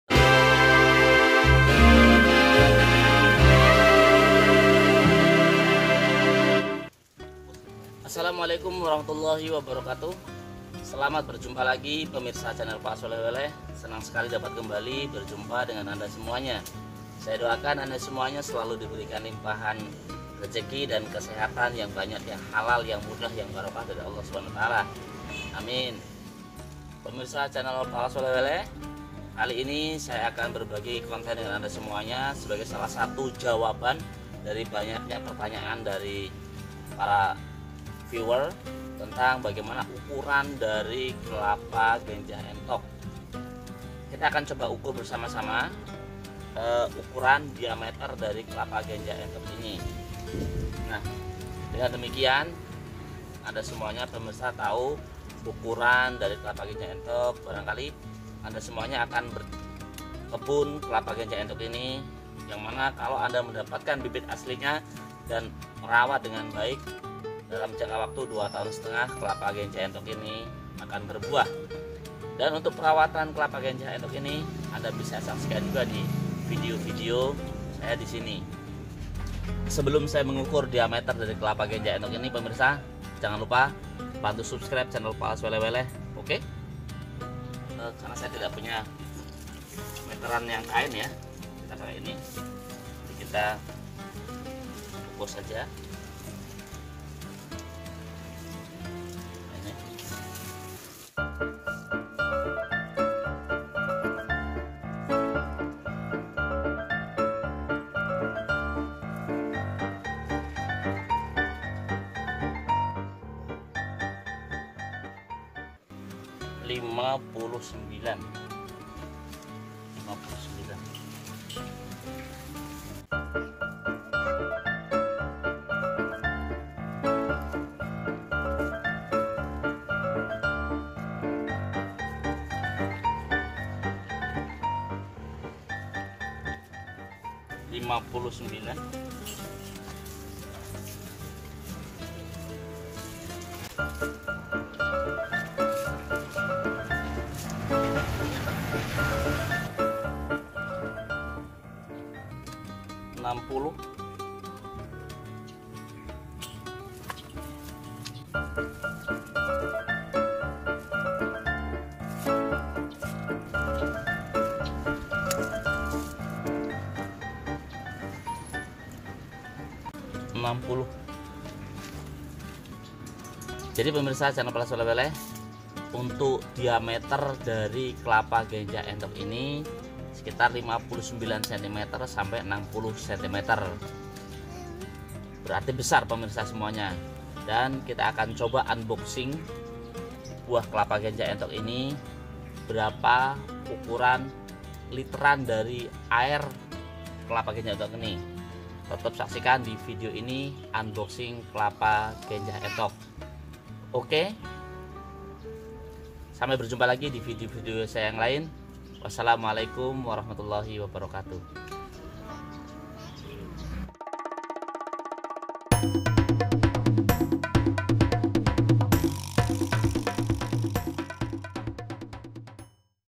Ukuran Diameter Buah Kelapa Genjah Entog
Yang Butuh Bibitnya Chat  ( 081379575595 )

Diameter Ukuran kelapa genjah entog. Monten ini sebagai jawaban dari para viewer yg bertanya ukuran dari kelapa genjah entog.
Yang saya ukur ini adalah buah awal dari kelapa genjah entog. Kami perkirakan ukuran buah kelapa genjah entog ini bisa lebih besar lagi.